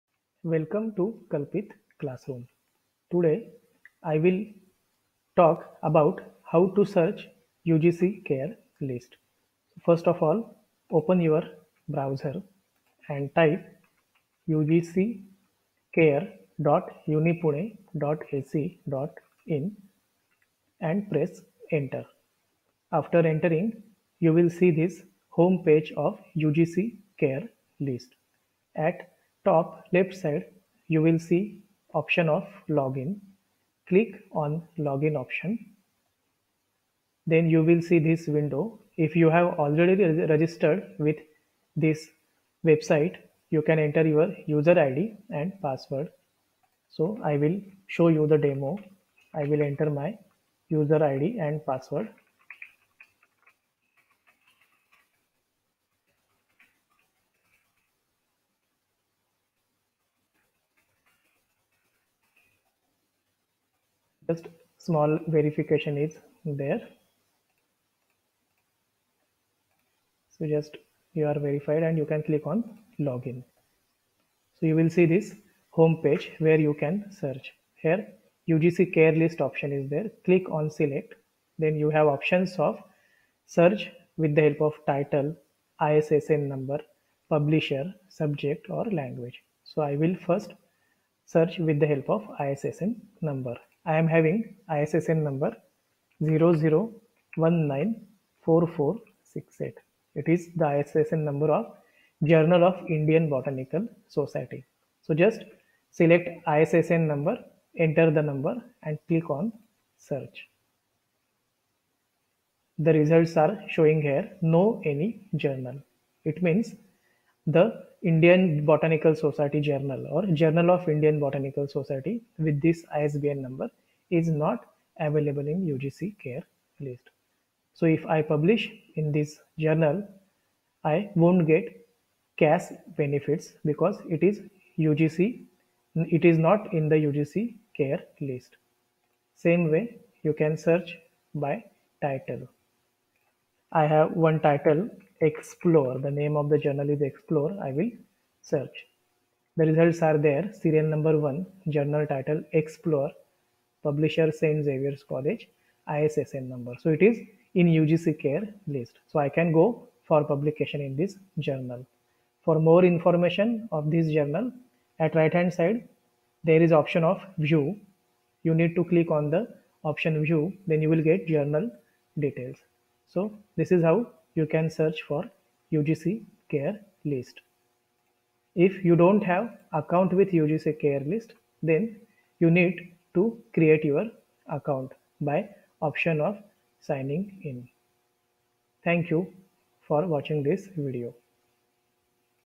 How to Search UGC care journal
This video will help teachers to find out UGC CARE journals.  Please confirm your journal before sending papers.  Here's I have made a small attempt to help find UGC CARE journals.